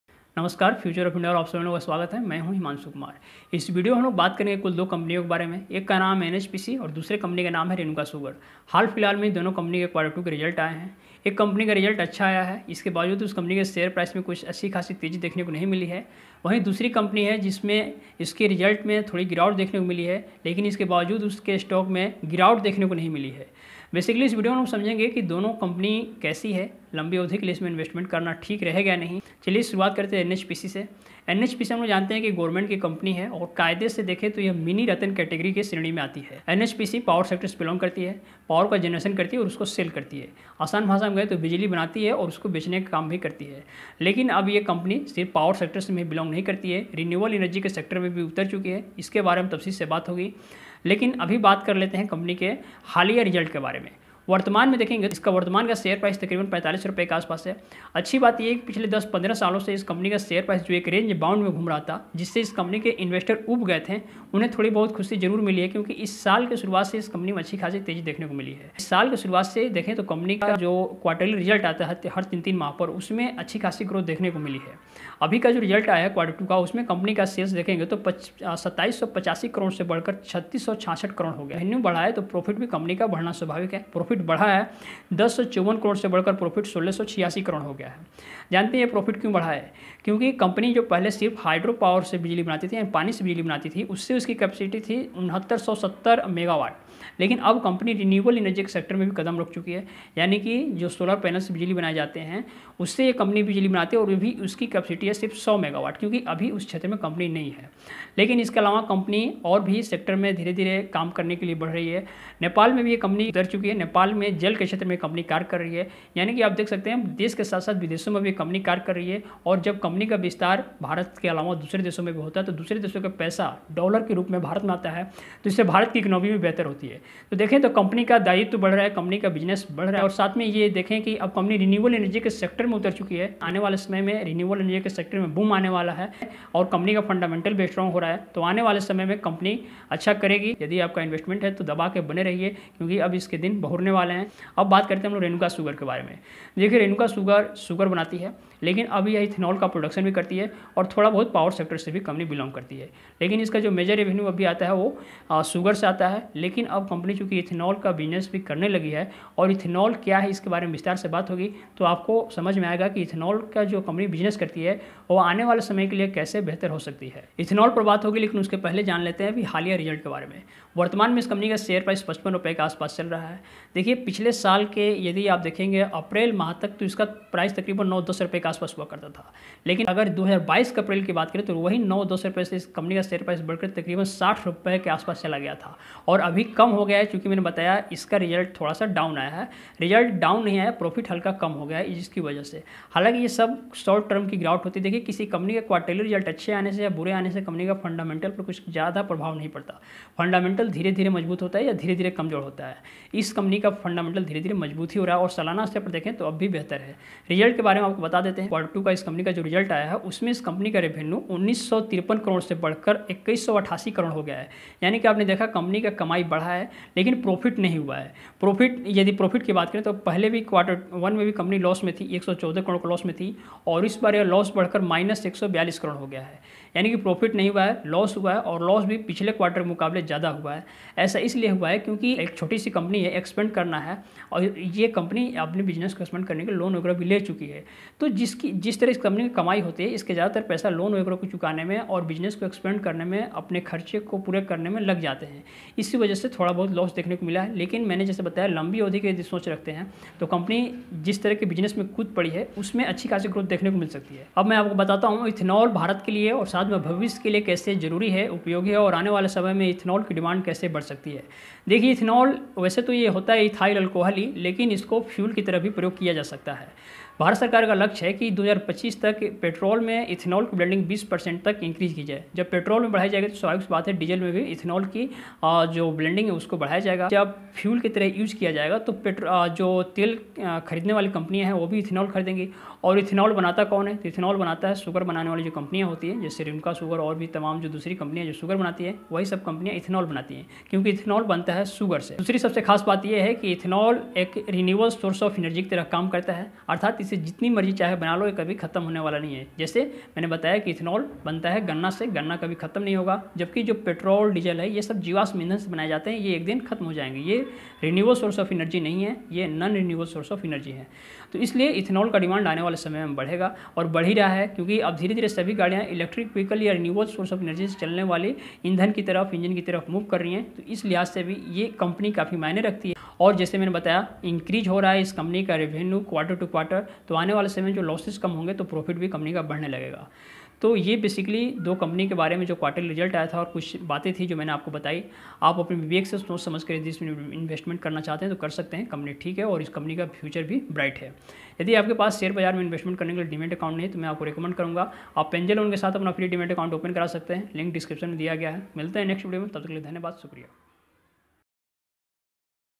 Nhpc share latest news । Renuka sugar share latest news । Future of India
In this video you will know about nhpc share and renuka sugar latest news and updates .

Open Free Demat Account in Angel One -

Renuka sugar
Renuka sugar ltd
Renuka sugar share news
Nhpc share latest news

Hash Tags -

Please Note:-